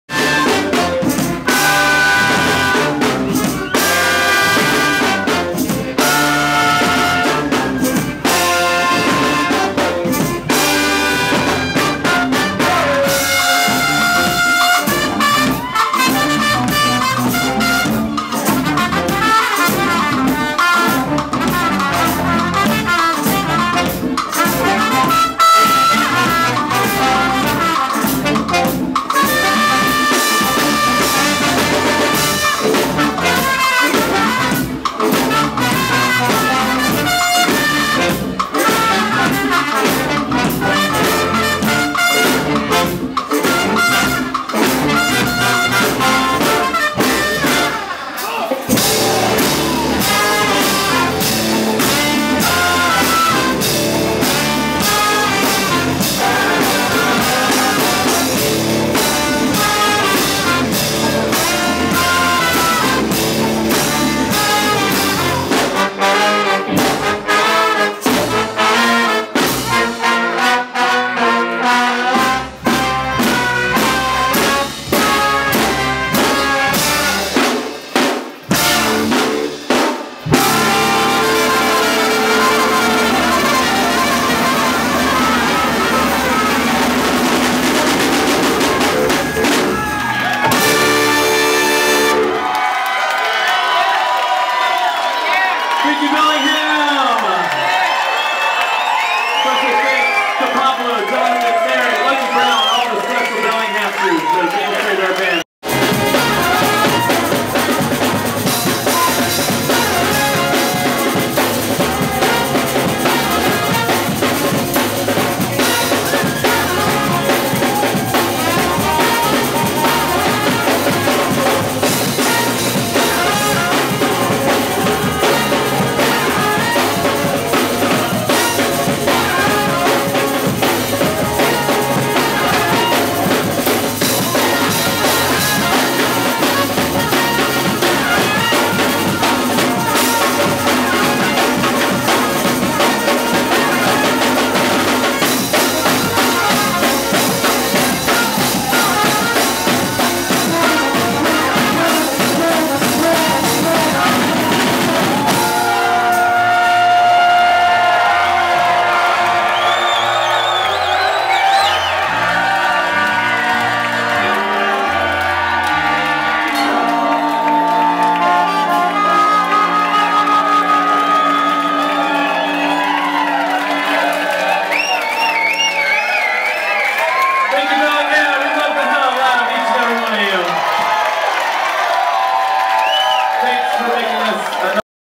March Fourth Marching Band @ Wild Buffalo
The March Fourth Marching Band (Portland, OR)

Quick video of the band playing and me sitting in on a solo!